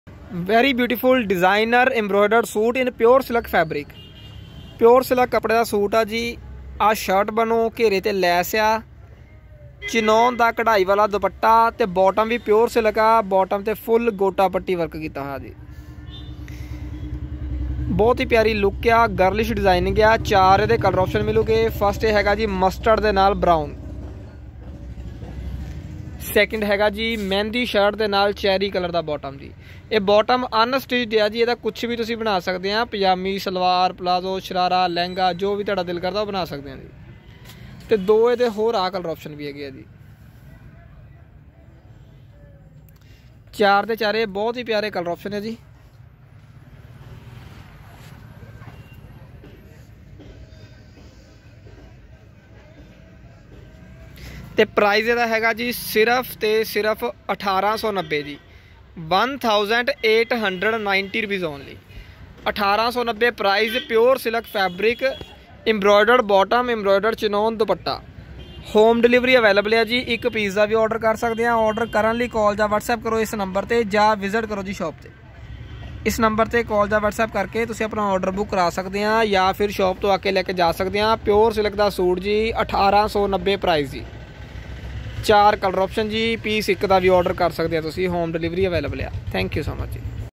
latest partywear punjabi suits Collection 2021 wedding suits silk suits at wholesale prices
Very beautiful boutique style suits in pure silk with beautiful designer embroidered chinnon duppata at lowest market price guranteed
Available at SARDAR fashion shop old bus stand chownk nurmahal district Jalandhar punjab serious buyers plz 🙏🙏🙏🙏
👗stitching available 🚛home delivery available ✈ shipping available